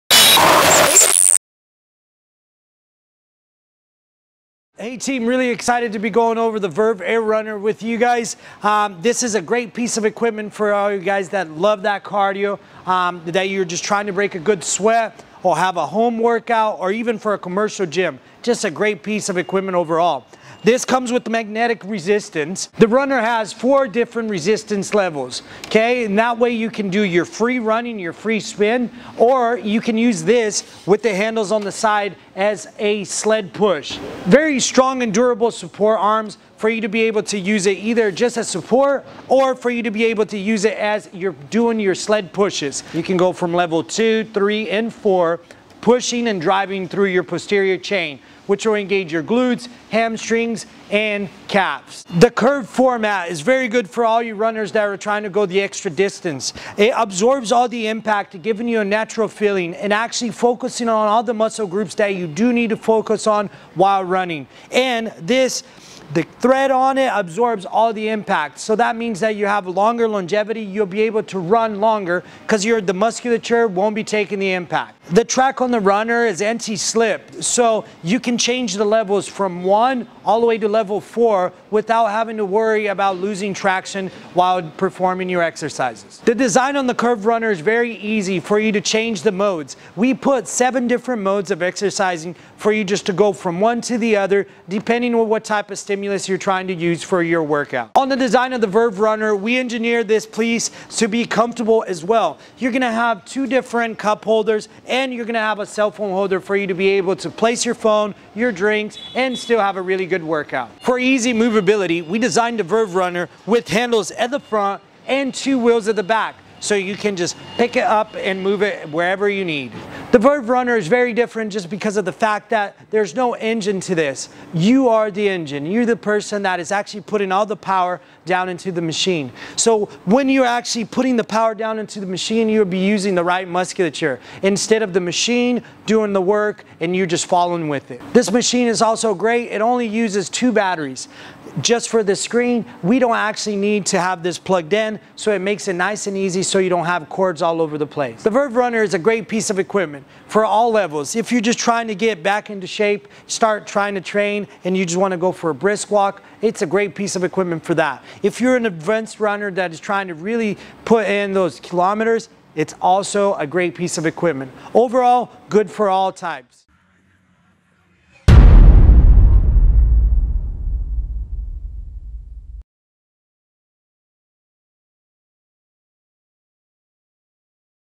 VERVE Air Runner with Magnetic Resistance
The VERVE Runner with sled function and magnetic resistance is the ultimate running machine allowing you a truly natural feel with the ability to easily adjust the intensity. Unlike standard electric treadmills, the VERVE Curved Treadmill features a curved magnetic running track that allows the athlete to free run or create a level of resistance for sled training - all power being created by the athlete for that true running training.

You are welcome to visit our new showroom at 4 Dalton Street, Upper Coomera, QLD, 4209 or phone us on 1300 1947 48 and see for yourself why we are Australia's #1 Fitness Equipment Brand.